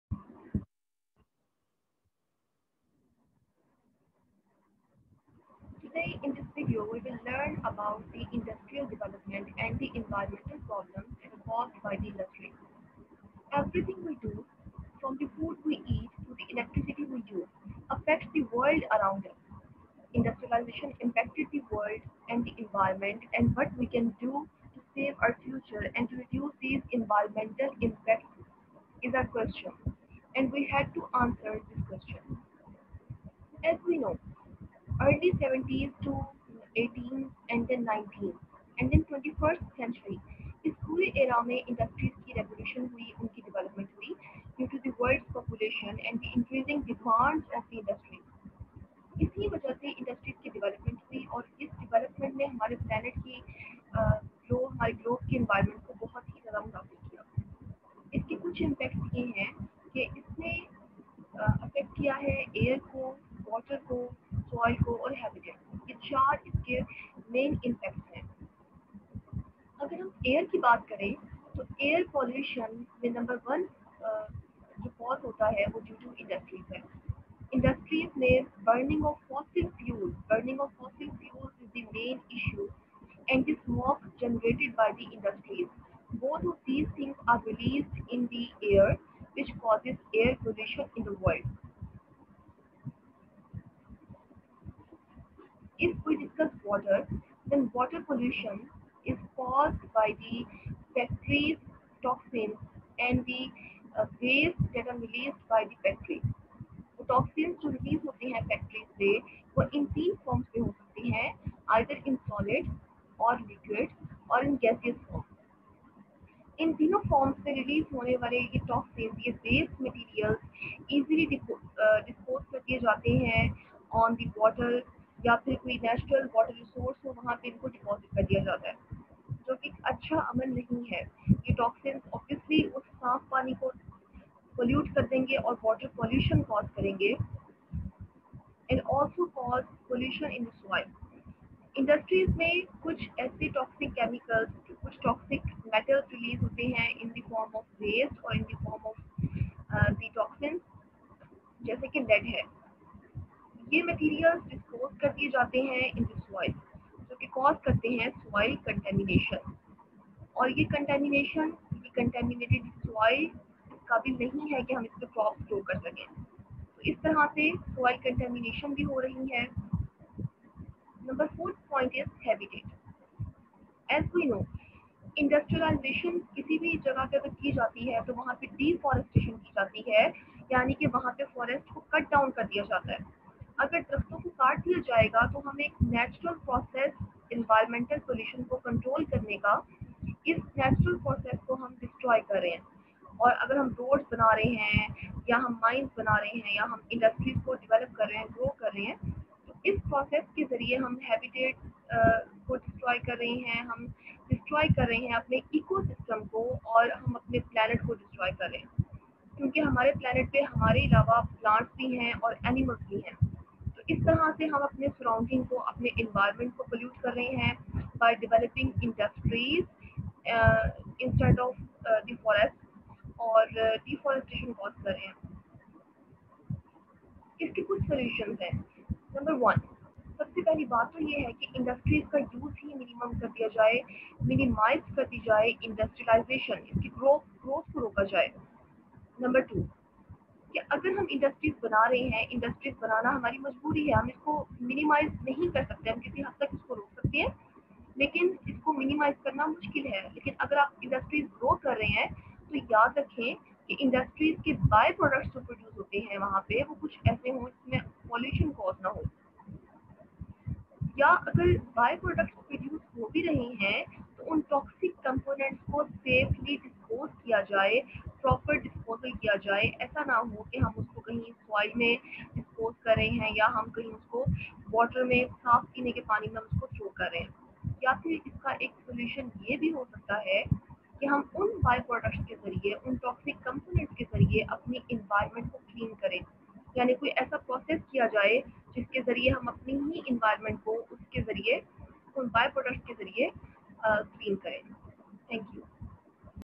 ADP Environmental science | Industrial Development and Environment | Impact of industries
In this video we will learn about how the growth of industries is affecting our planet. Everything we do from the food we eat to the electricity we use affects our world. industrialization impacted the environment and what can we do in the future to reduce these impacts.